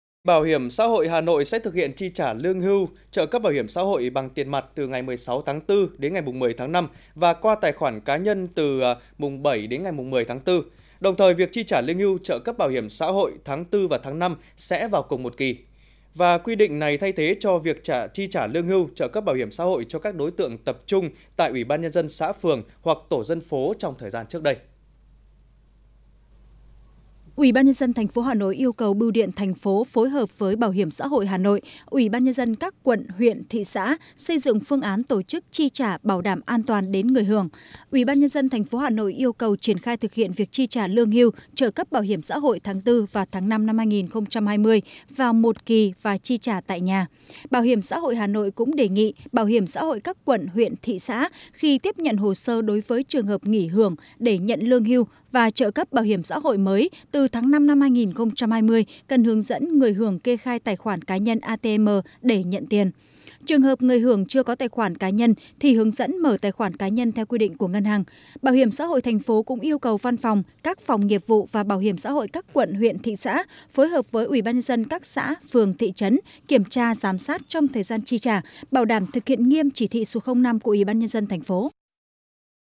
Hà Nội trả lương hưu, trợ cấp BHXH qua tài khoản ATM từ 7-4
Hà Nội trả lương hưu, trợ cấp BHXH qua tài khoản ATM từ 7-4 | Truyền Hình Nhân Dân
2. Dự Báo Thời Tiết Hôm Nay